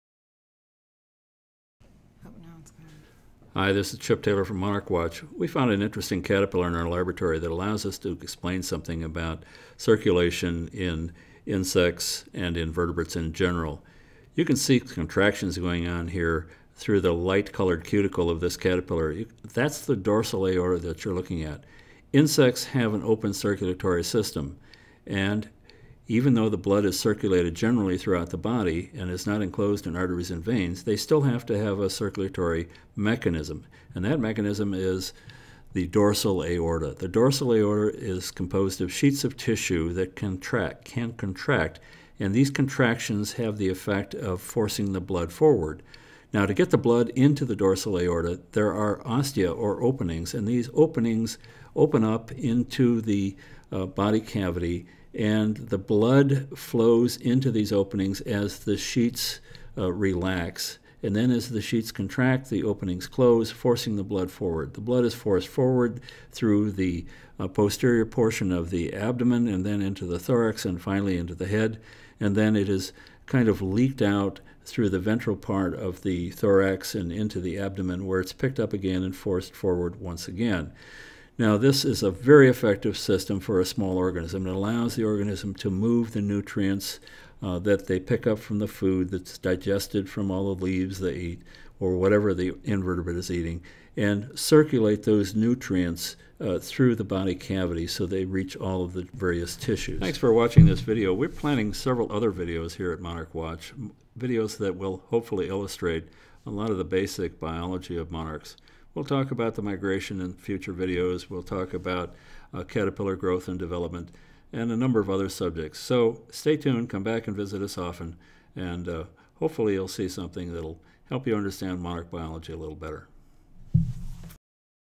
Monarch Caterpillar Dorsal Aorta
Serendipity = making fortunate discoveries. Occasionally, there is a little serendipity in the lab. One Friday, a few weeks ago, I noticed an unusual larva, a fourth instar of a "black"  larval mutation we are studying. This particular larva was lighter than most and we could see the blood coursing through the dorsal aorta. I said "Let's get a camera, this is neat!" In the course of doing the filming, and not doing it well, I kept getting advice from one of our critter crew members Alicia Bigelow, and I soon realized (another bit of serendipity) that Alicia needed to be in charge of the production of video projects that I've always wanted to post to Monarch Watch's YouTube channel. So, here is the first production: The dorsal aorta. In the voice-over I describe the general pattern of blood circulation in insects and arthropods. Chip Taylor | Director, Monarch Watch